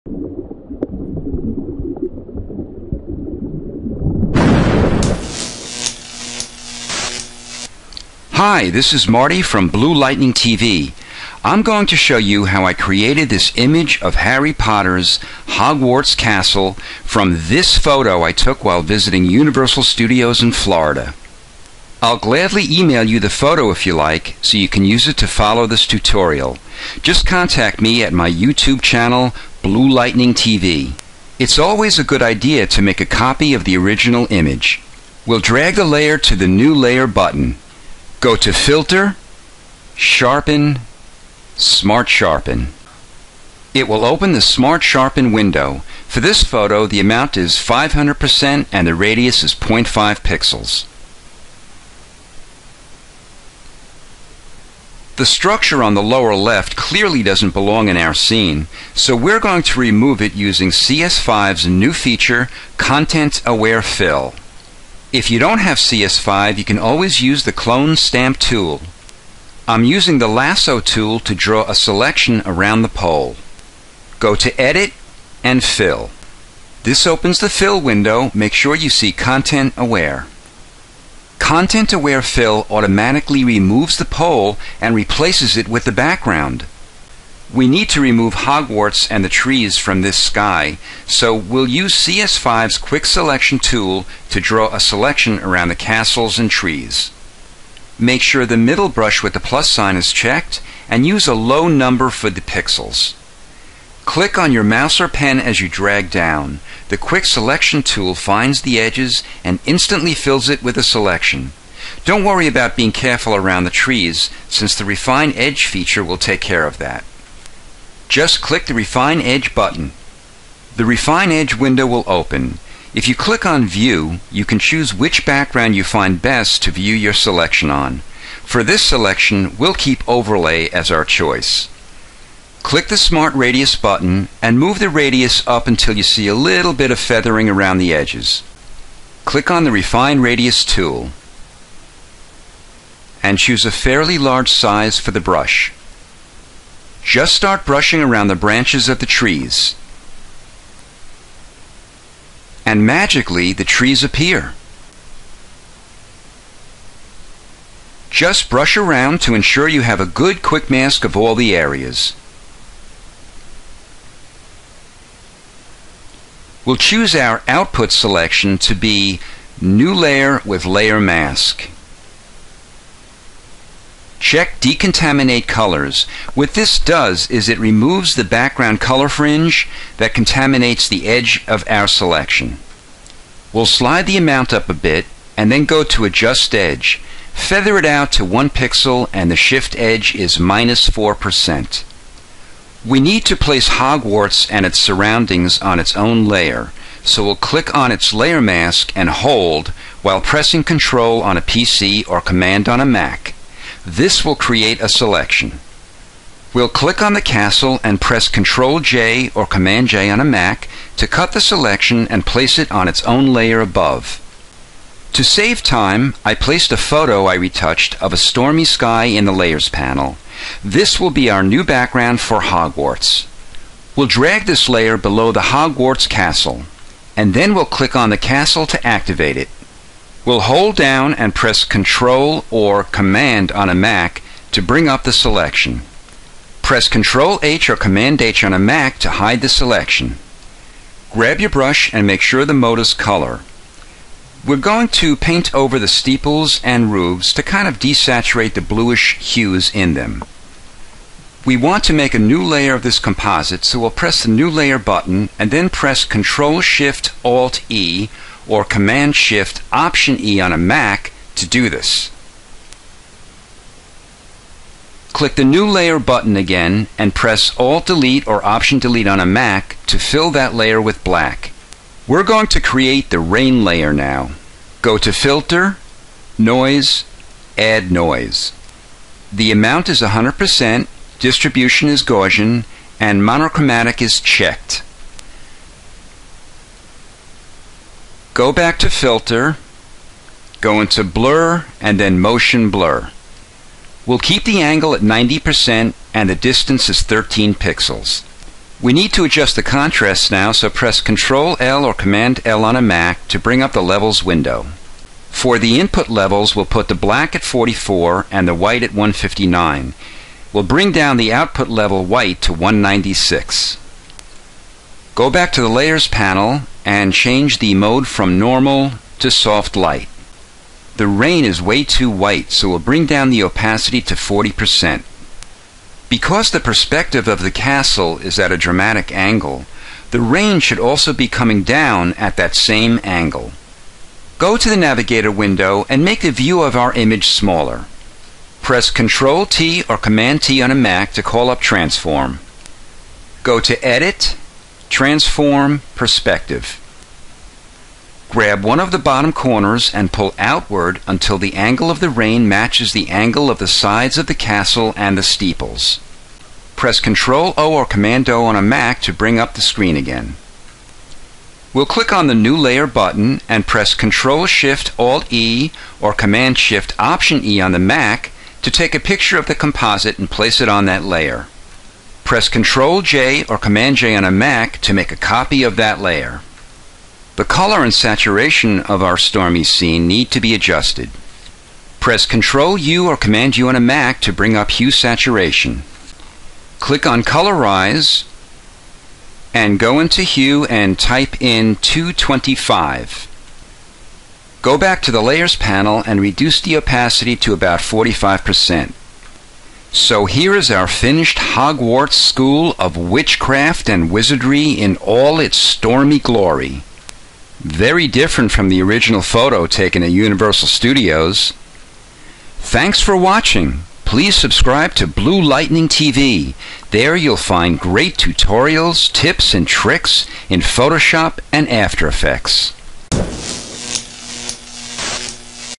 Photoshop Tutorial: How to Make a RAIN Storm!
HD Photoshop tutorial showing how to create a photo-realistic rain storm effect.